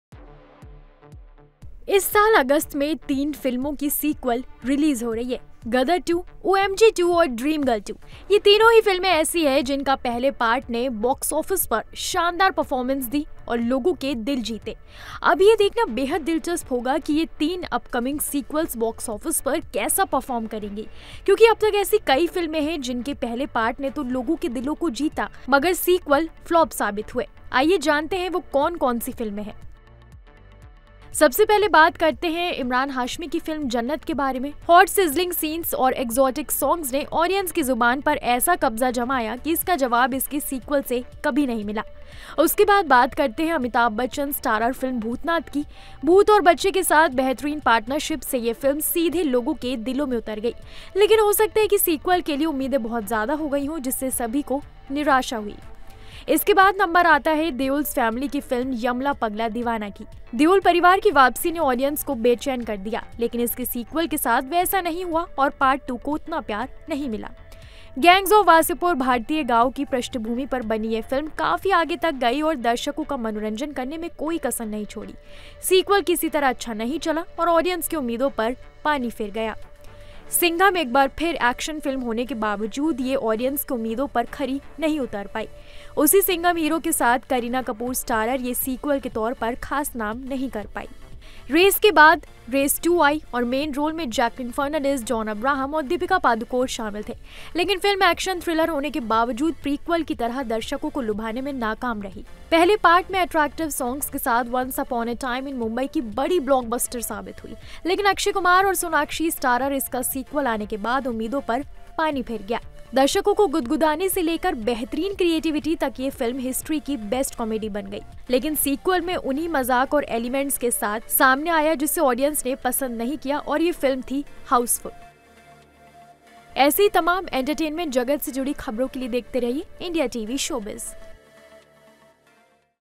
Bollywood movies with flop sequels| Entertainment News| Bollywood Films
Sequels of three films are releasing in August this year.. Gadar 2, OMG 2 and Dream Girl 2...these three films are like this..whose first part gave a great performance at the box office and won the hearts of people..now this It will be very interesting to see how these three upcoming sequels will perform at the box office..because till now there are many such films whose first part won the hearts of the people but the sequel proved to be a flop..
Let us know which are those films.

इस साल अगस्त में तीन फिल्मों के सीक्वल रिलीज हो रहे हैं.. गदर 2, ओएमजी 2 और ड्रीम गर्ल 2.. ये तीन फिल्में इस तरह हैं.. जिनके पहले पार्ट ने बॉक्स ऑफिस पर शानदार प्रदर्शन किया और लोगों का दिल जीता। .अब ये देखना बेहद दिलचस्प होगा कि आने वाले ये तीनों सीक्वल बॉक्स ऑफिस पर कैसा प्रदर्शन करेंगे.. क्योंकि अब तक कई ऐसी फिल्में हैं जिनके पहले पार्ट ने तो लोगों का दिल जीत लिया लेकिन सीक्वल फ्लॉप साबित हुआ..
आइए जानते हैं कौन सी हैं वो फिल्में.